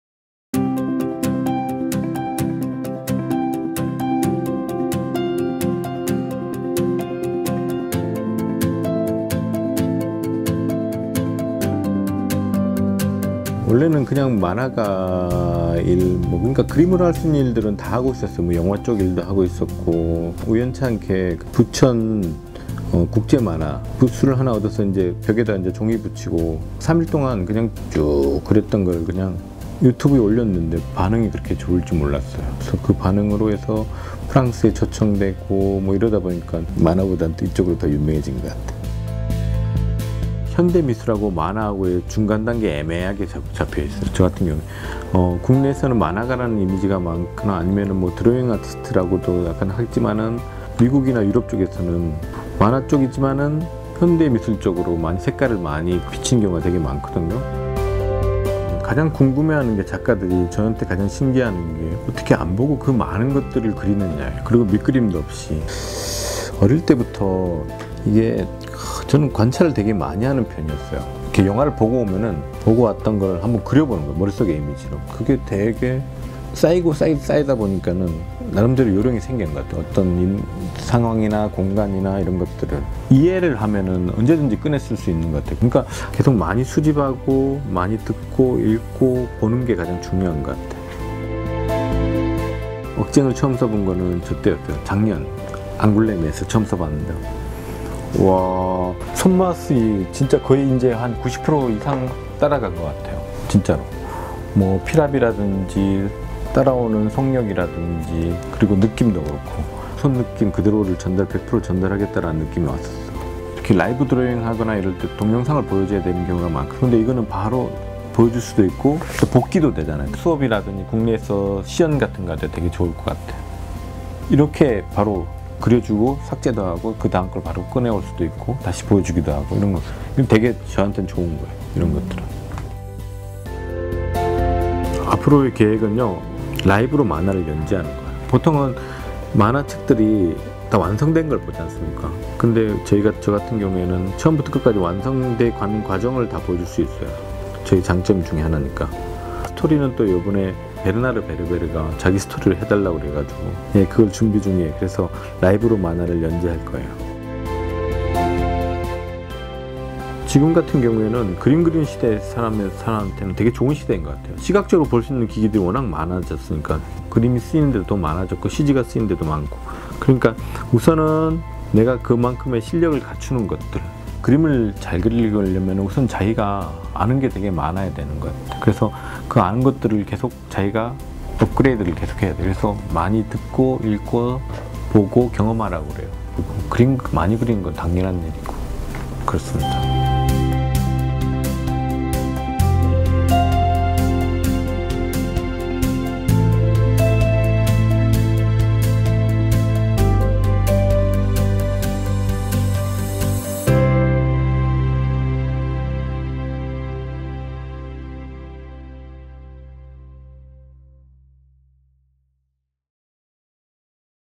[와콤 아카이브ㅣ함께한 크리에이터] Kim Jung Gi, 'God of live drawing art'ㅣ2016 와콤 크리에이티브 세미나
Kim Jung Gi (Cartoonist, drawing artist)
2002 Debuted with the cartoon 'Funny Funny'
2010 Contributed the comic series TLT (Tiger the Long Tail) to Naver, No. 1 portal in Korea
2010 Illustrated for Bernard Werber's novel "Paradise"
2011 Held a drawing show at Bucheon International Comics Festival
2013 Illustrated for Bernard Werber's novel 'Third Humanity'

2014 Made his debut in France with the comic book "Spy Games'
Held a Pop-Up Art Show in New York
2015 Participated in Angoulême Festival International de la Bande Dessinée
Participated in Murakami Takashi's ‘GEISEI x infinity’
Held his personal exhibition ‘Kim Jung Gi x Drawing’

From a cartoonist to 'god of live drawing', Jung Gi Kim covers various types of art from cartoon, commercial & movie production to modern art segment. In this video, he shares the story of his life as an artist, his know-how of live drawing and his workflow. Check out his message to the artists all around the world!